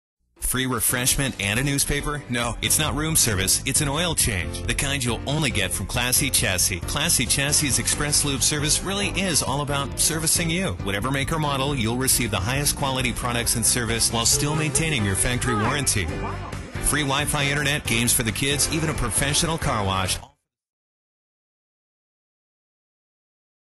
Classy Chassis Xpress Lube Oil Change
Lakewood's Drive-thru oil change service! Our Chevron Xpress Lube services all makes, is open 7 days, and no appointment is required! FREE car wash with EVERY oil change! AOCA certified techs.